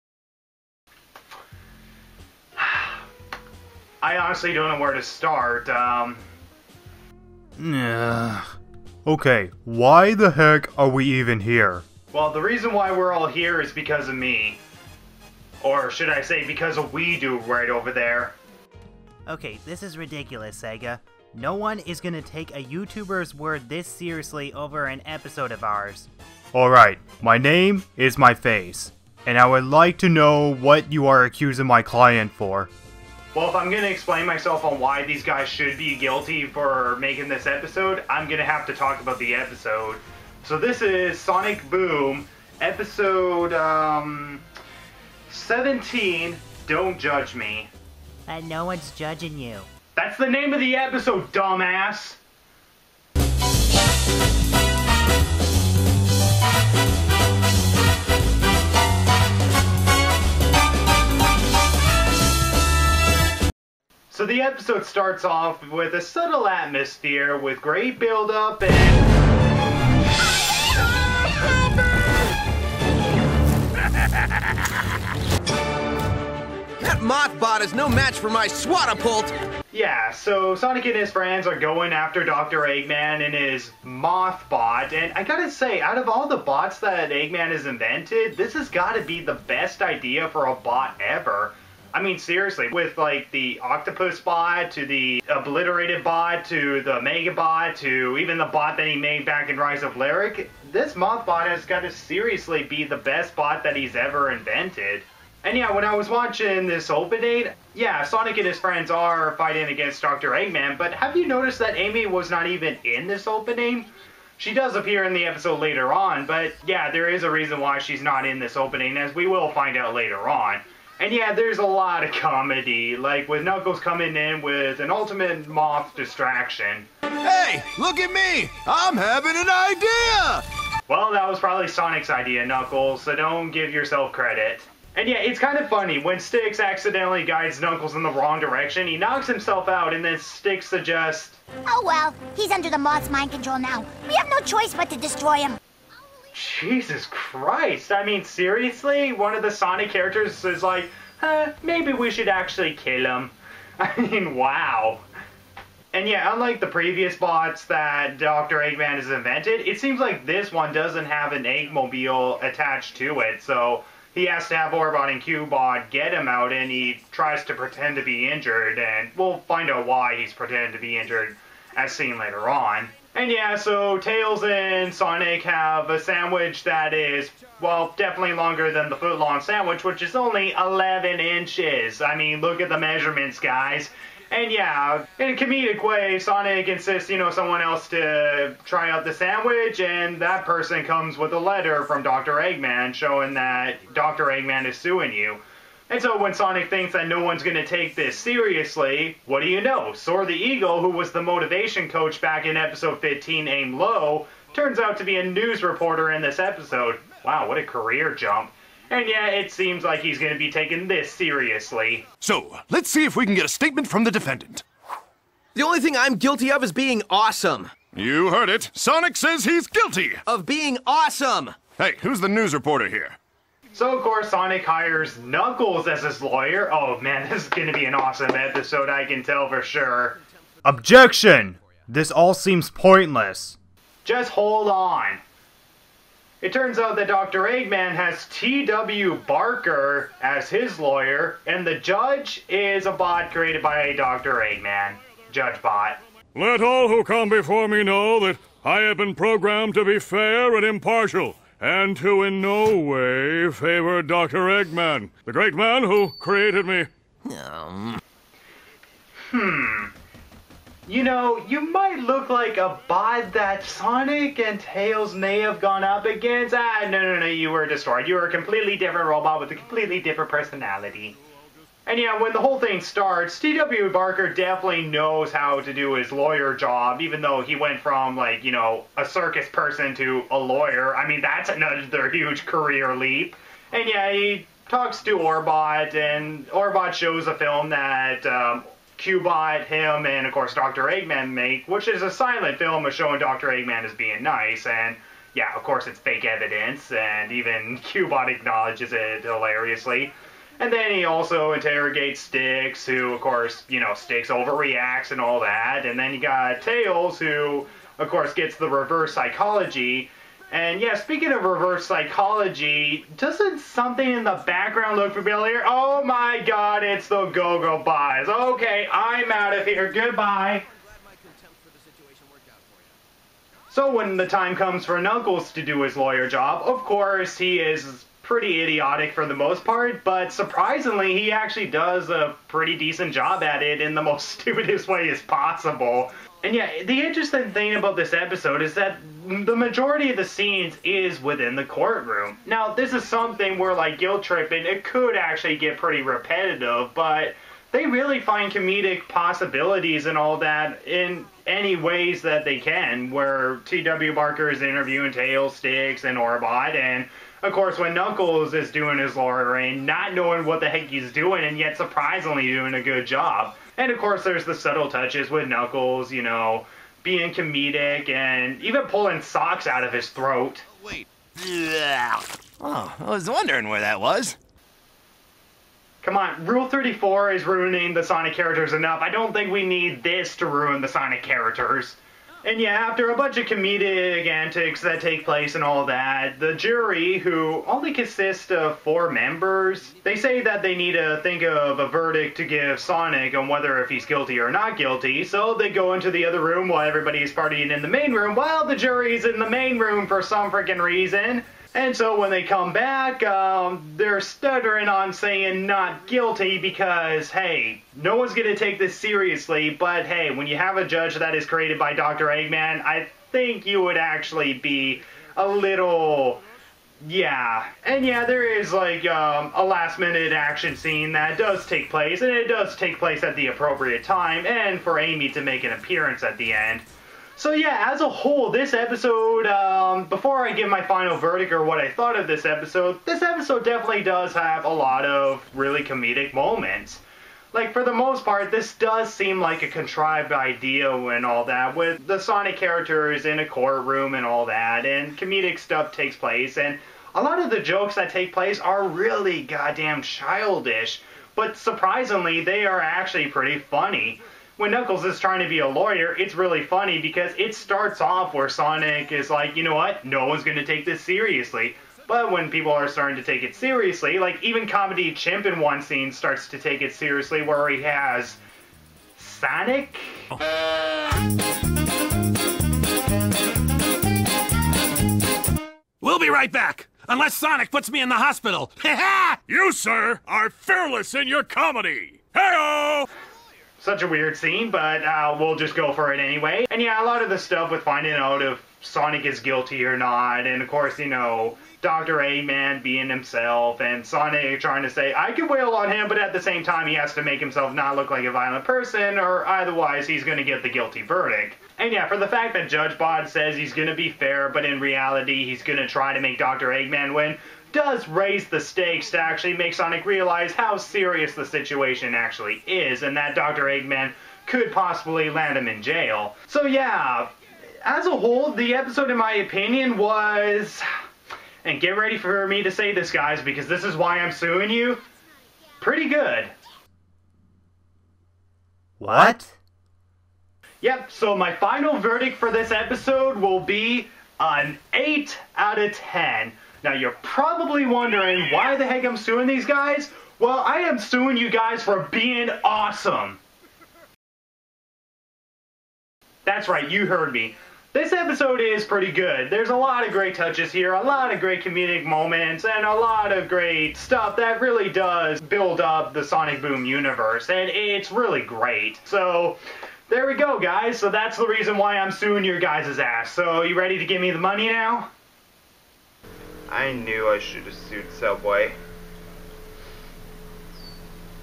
Sonic Boom Vlogs - Episode 17 - Don't Judge Me
This episode has a lot of interesting stuff to it, OuiDo Productions should be sued for making it! After Sonic and his friends defeat Dr. Eggman in his Moth Bot, he sues Sonic for his injuries. Sonic knowing that everyone knows who he is and that no one will take this trial very seriously, he hires Knuckles to be his lawyer who has absolutely no experience with this job position. Heck he doesn't know which end of the toothbrush goes into your nose. But when the judge turns out to be Burnbot created by Dr. Eggman himself, the stakes all rely on not making Sonic look like a psychopath.